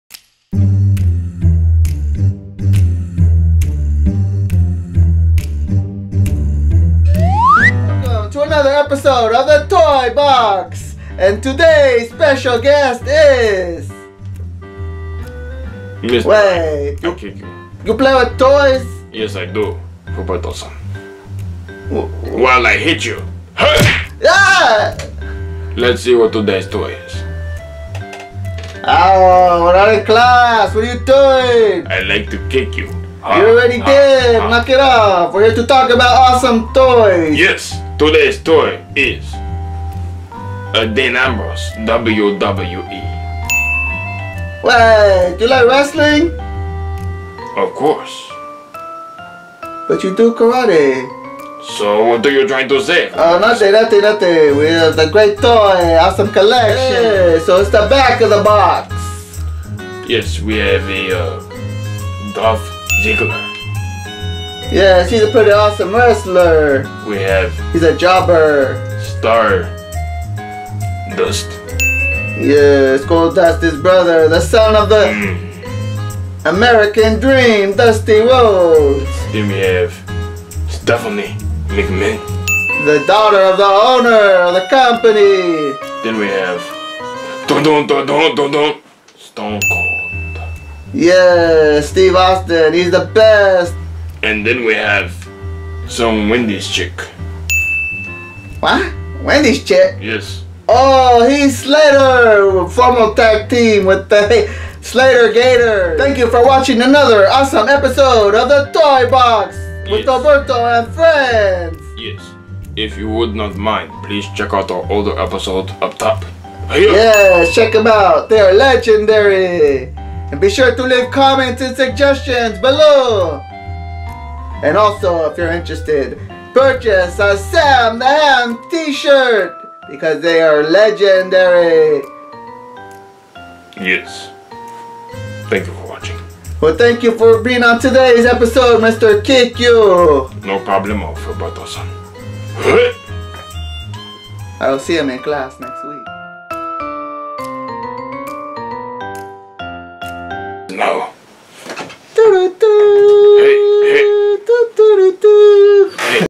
WWE DEAN AMBROSE ACTION FIGURE (PHILBERTO'S TOY BOX ) | Sam The Ham
Check out this week's toy as we review the WWE Dean Ambrose Action Figure !